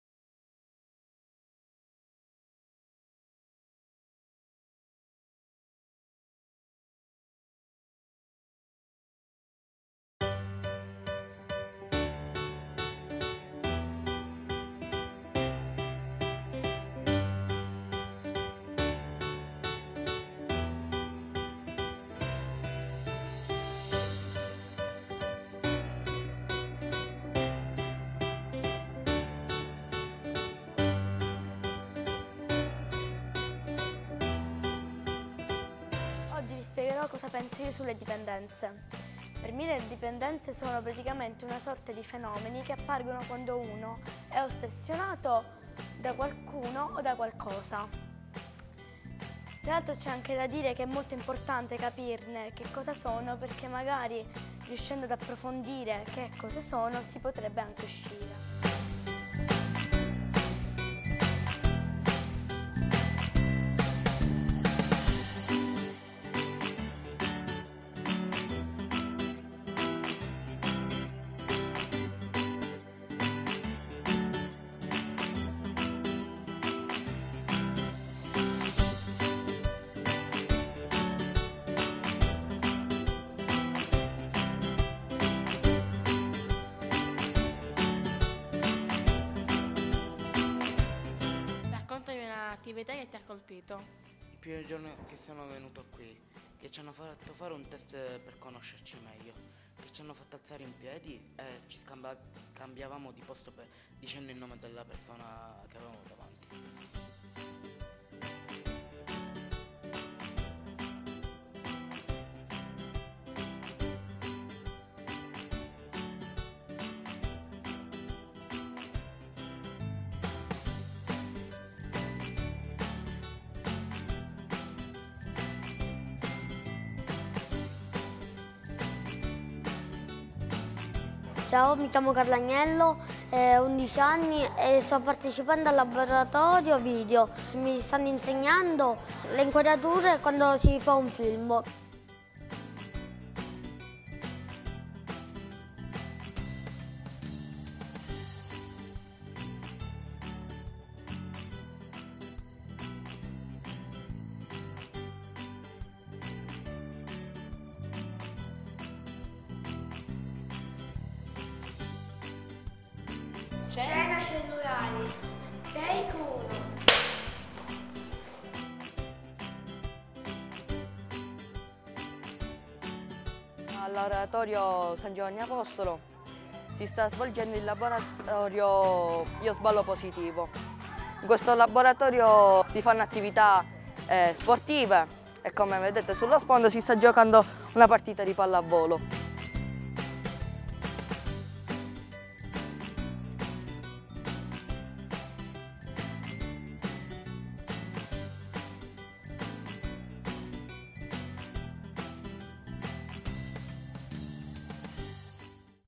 Promo del Progetto "Io sballo positivo"
Al via il Progetto "Io sballo positivo" contro l’abuso di alcool, droghe, vecchie e nuove dipendenze presso l’Istituto onnicomprensivo Giuliana Saladino, capofila , in collaborazione con l’Associazione A.F.I.Pre.S. Marco Saura e S.Giovanni Apostolo.
L’iniziativa, finanziata dall’Assessorato della Famiglia, delle Politiche sociali e del Lavoro della Regione siciliana per promuovere il benessere dei giovani a rischio, si volge negli spazi dell’Istituto comprensivo statale Giuliana Saladino.
Un intervento di prevenzione dell’uso di alcool, di fumo e di tutte le nuove dipendenze patologiche ancora poco conosciute che coinvolgerà alunni, docenti, operatori del territorio e famiglie. Soggetti con i quali si sta costruendo un percorso integrato, al fine di promuovere il benessere psico-fisico dei giovani attraverso azioni di accoglienza, ascolto, consulenza, laboratoriali, operative ogni settimana nei locali della scuola, e con la realizzazione di uno spot.
Docenti, genitori, ragazzi, con la guida di Esperti A.F.I.Pre.S. quali la Psicoterapeuta Viviana Cutaia, la Psicodrammatista Rosalia Billeci, le Video Makers Elisa Calunniato e Mariapia Rizzo, ed Esperti S.Giovanni Apostolo, costruiranno percorso di promozione della salute.